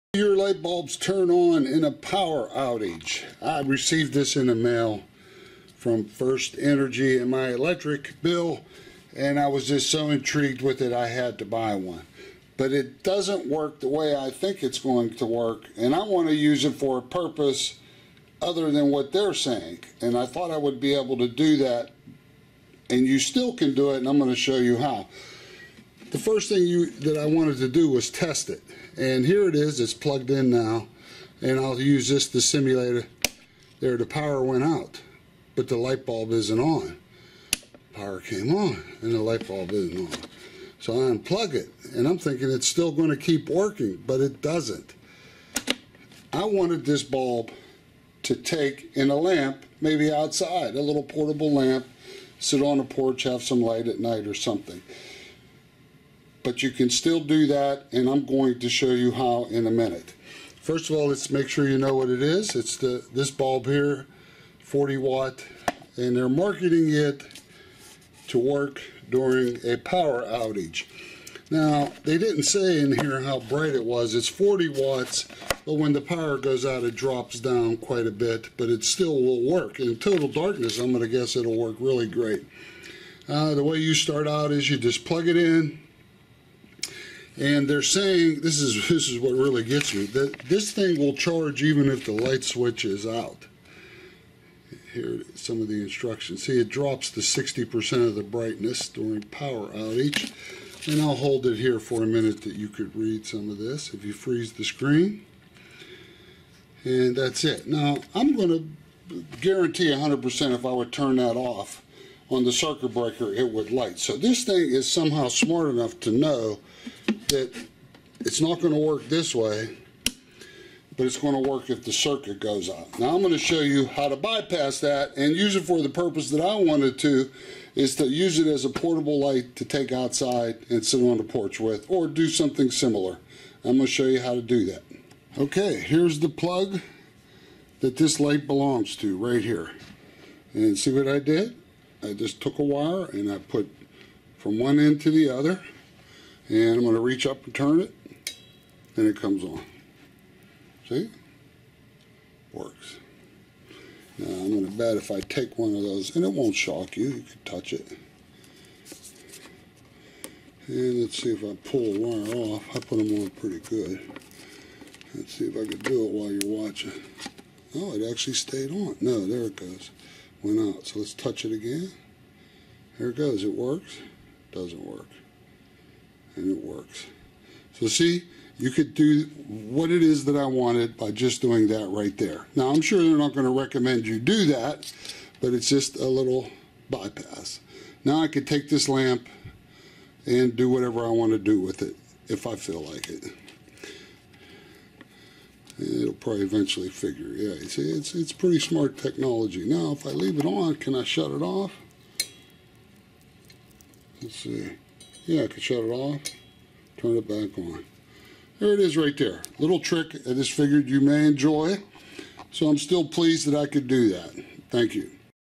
Power Outage Sengled Bulb Bypass Trick (smart light bulb)
FirstEnergy incldued an offer in my electric bill to purchase a light that would work during a power outage. the cost was $20.00 delivered. I thought I would purchase one to make a portable light. Problem was it does not light when unplugged. In this video I will show you how to bypass all of that and make it a portable light.